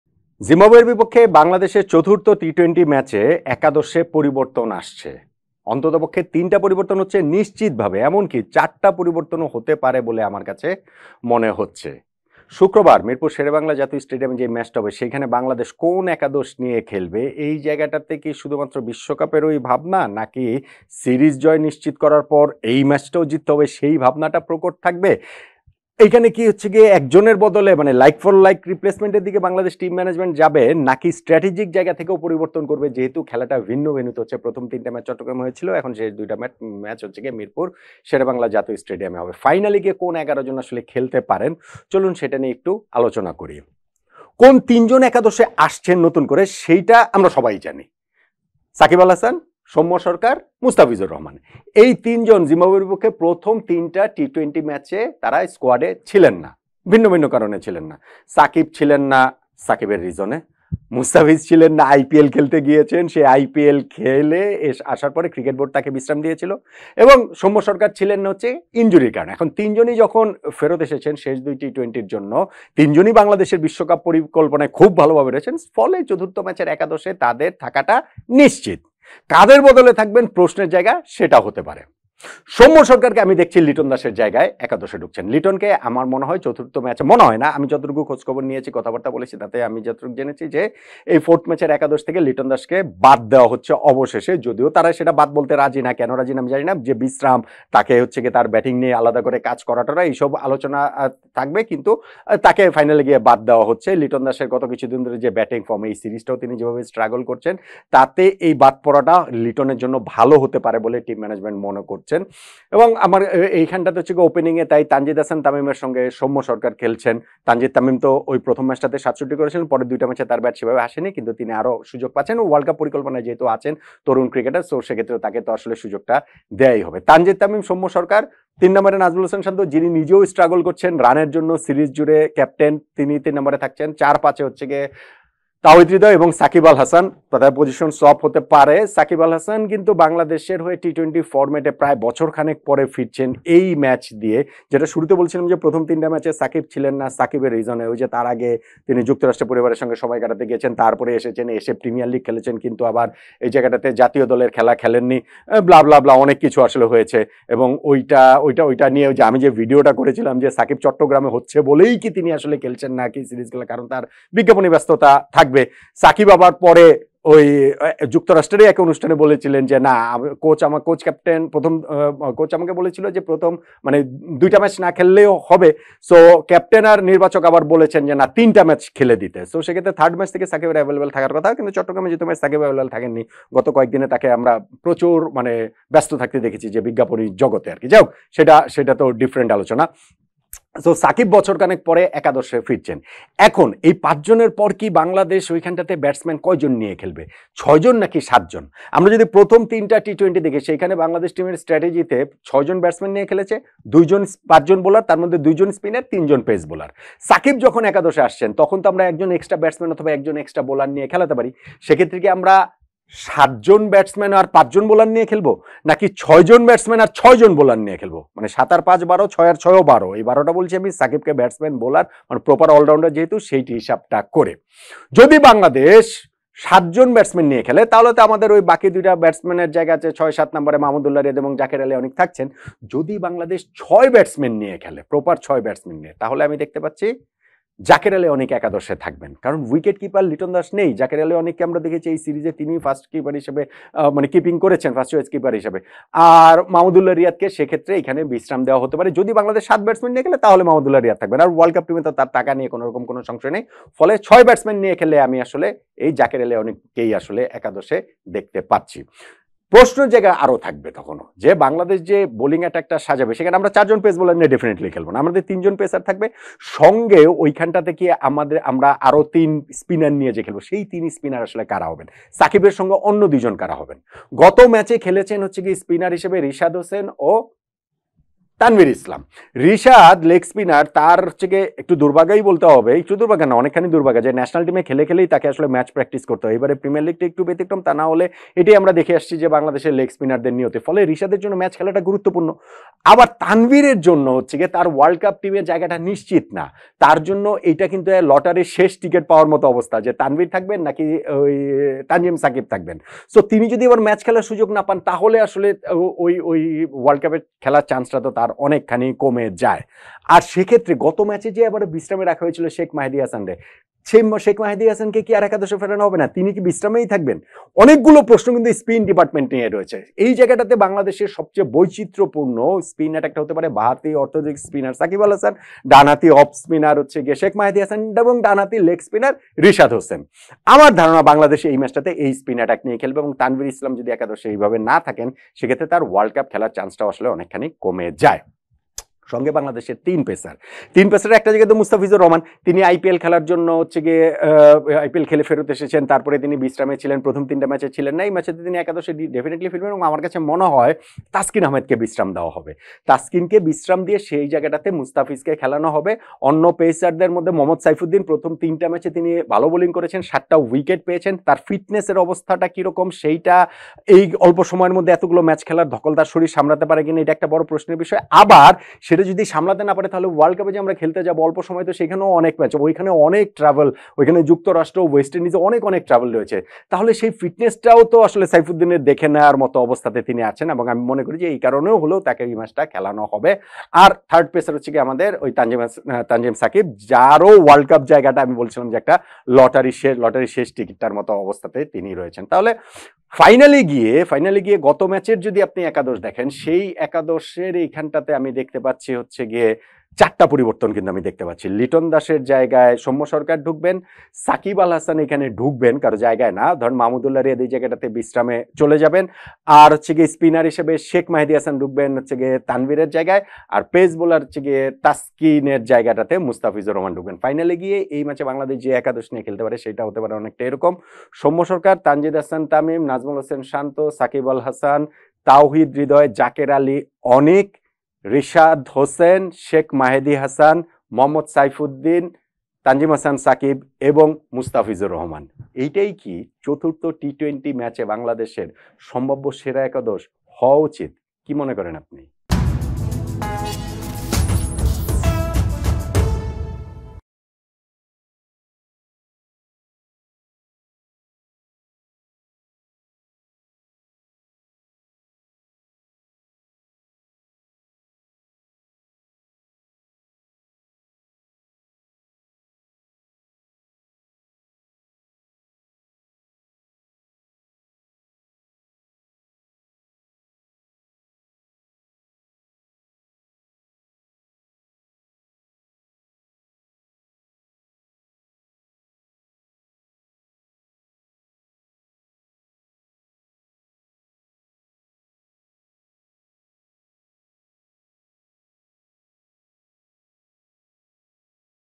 ৪ পরিবর্তনের একাদশে লিটন বাদ অবশেষে? – নট আউট নোমান
জিম্বাবুয়ের বিপক্ষে চতুর্থ টি-টোয়েন্টিতে বাংলাদেশ একাদশে পরিবর্তন হবে। অন্তত ৩টি পরিবর্তন তো নিশ্চিত; ৪টিও হতে পারে। কার আসছেন, কারা যাচ্ছেন? লিটন দাস কি বাদ পড়ছেন অবশেষে? নট আউট নোমান-এ সে আলোচনাই।